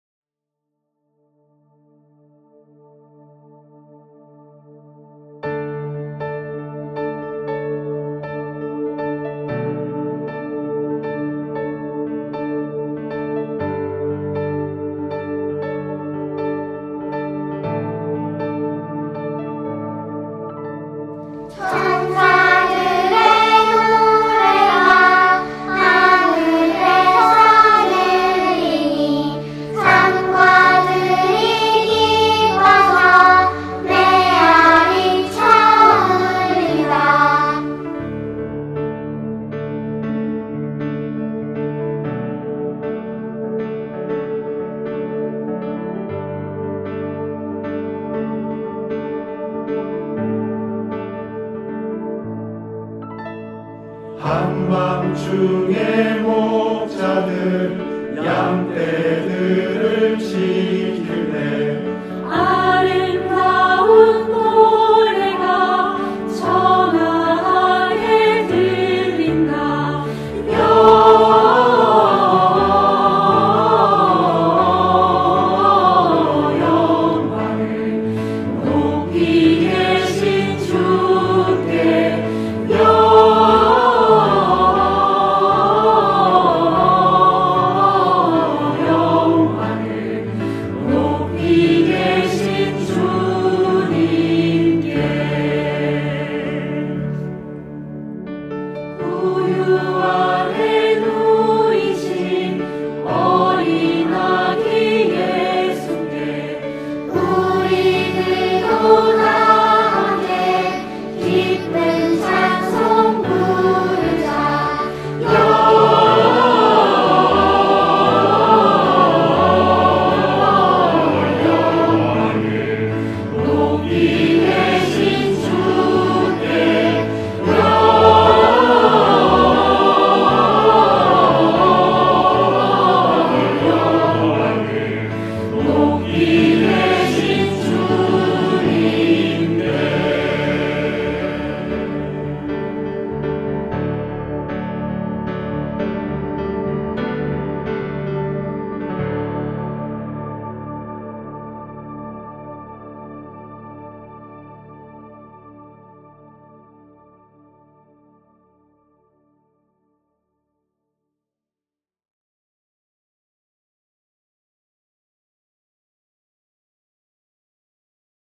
[소망교회] 버추얼콰이어 / 107인의 찬양대
이 영상은 소망교회 성탄절 봉헌송을 위해 제작된 영상입니다.
소망의 성도들 107명이 각자의 자리에서 찬양한 영상을 편집하여 버추얼콰이어로 재구성하였습니다.